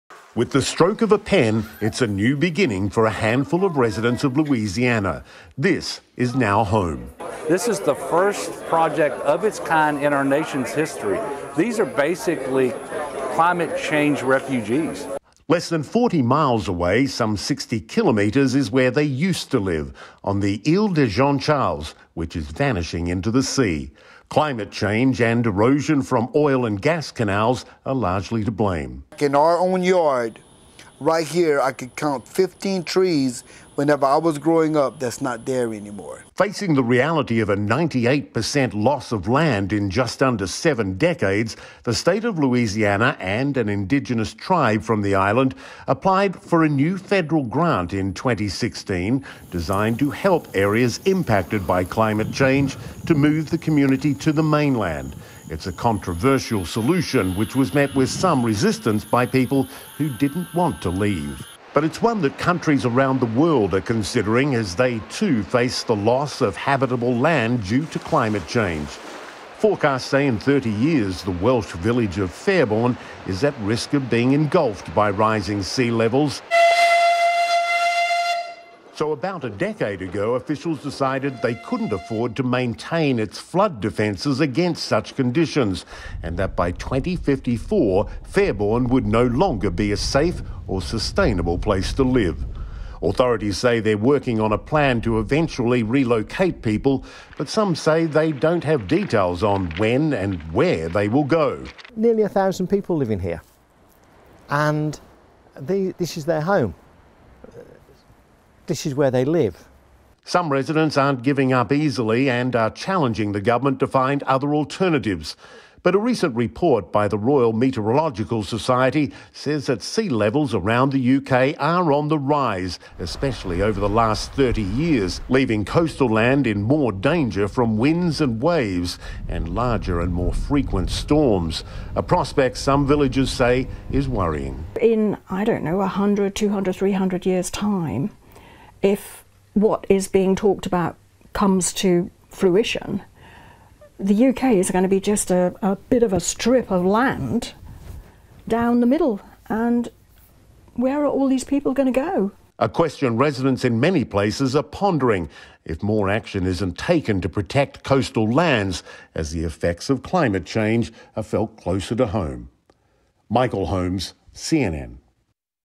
Island could lose 98% of land from climate change See how residents are responding
cnn Island could lose 98% of land from climate change. See how residents are responding

As climate change poses a threat to habitable land across the world, a federal resettlement grant has relocated some indigenous residents of a Louisiana isle to the mainland because their town is under threat of being submerged by water. CNN's Michael Holmes reports. Source: CNN